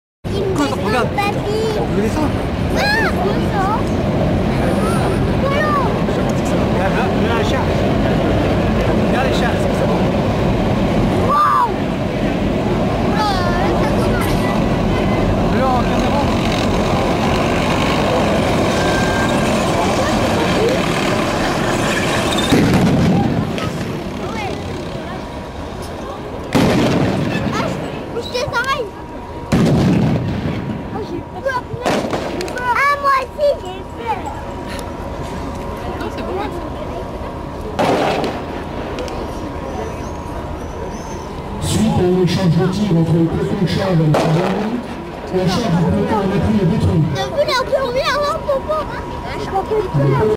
JPO CENTAC Mailly 2012 -4
Démonstration dynamique, Journées Portes Ouvertes CENTAC de Mailly le Camp, juin 2012.